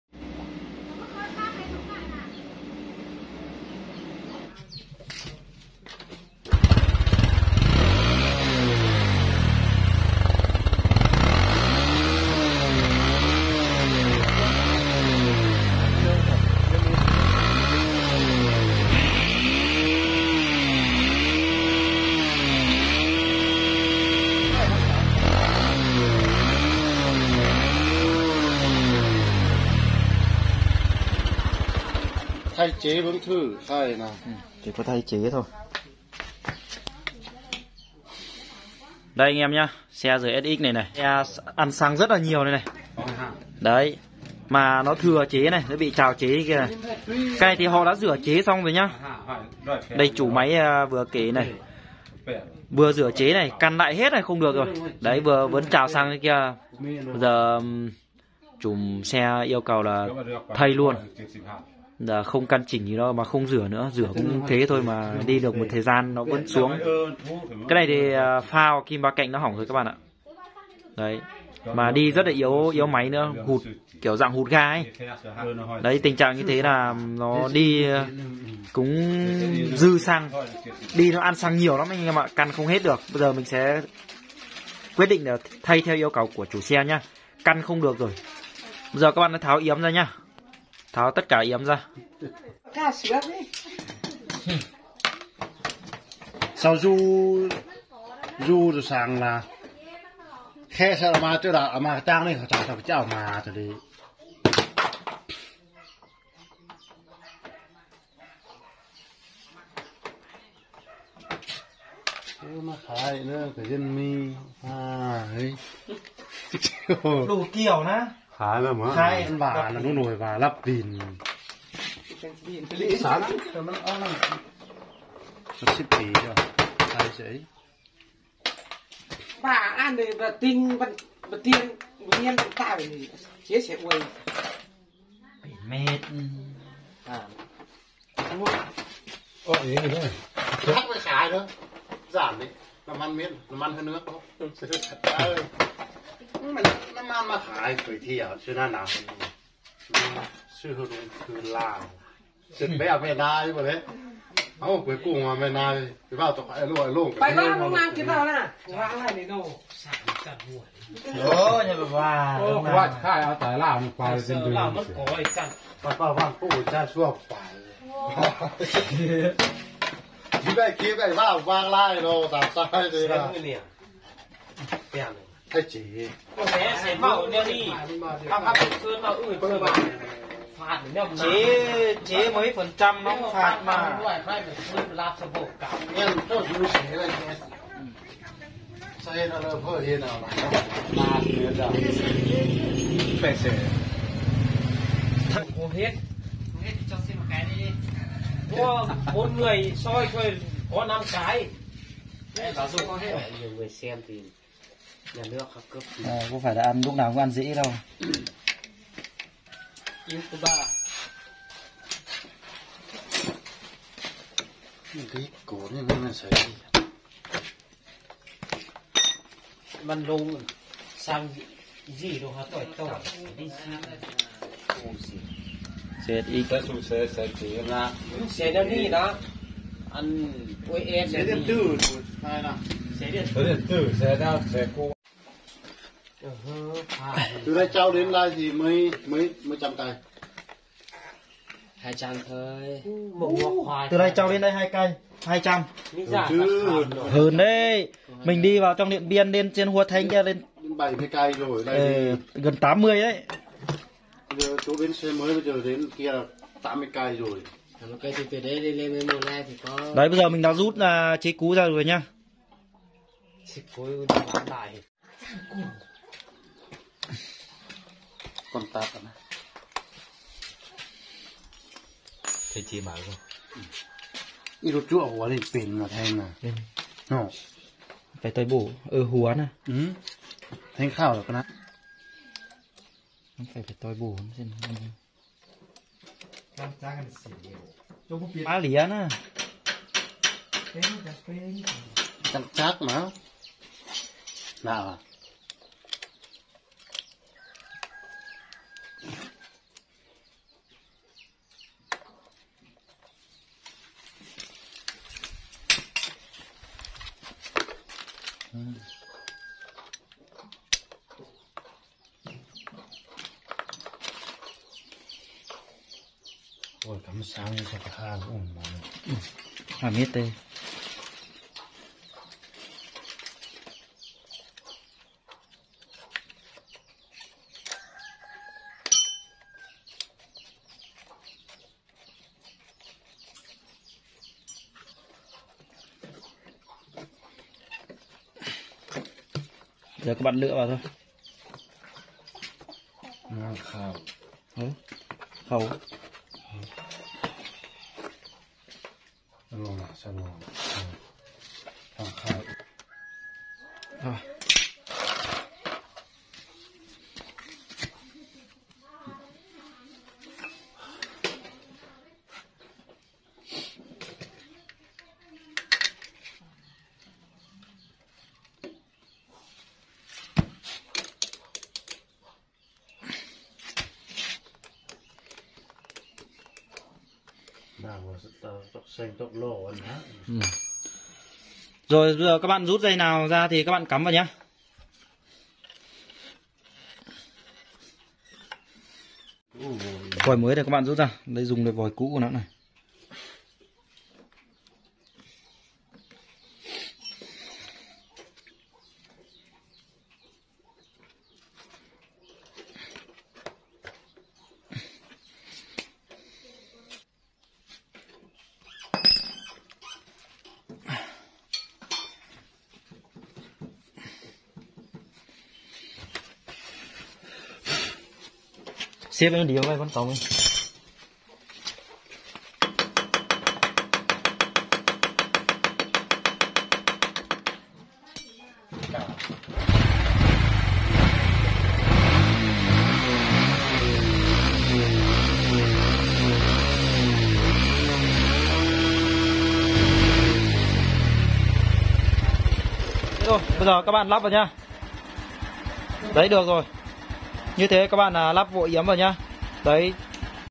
Đầu Nổ TV.thay chế xe honda RSX
Hôm nay mình sẽ chia sẻ với các bạn về bệnh xe máy Honda RSX bị chào xăng và ăn săng rất nhiều các bạn chú ý xem nhé